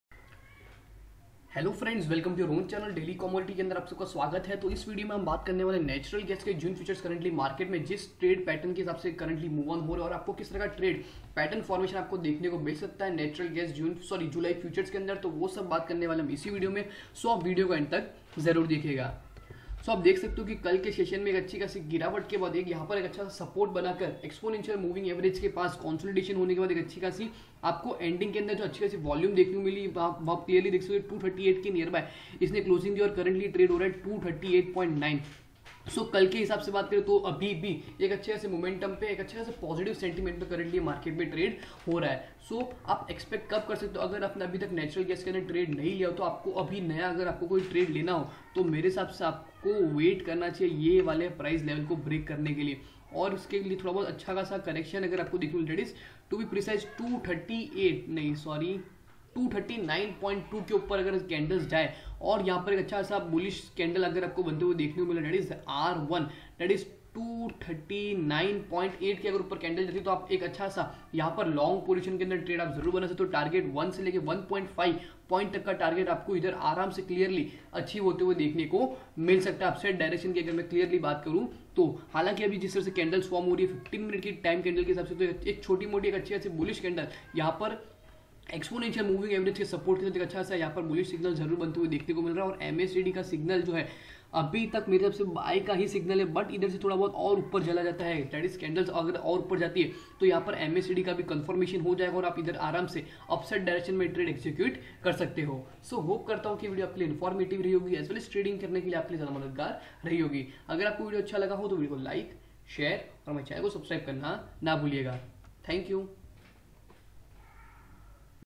Natural Gas Commodity Technical Chart | Natural Gas Trading July| Daily Commodity
Source: Commodity.com
Charts: Tradingview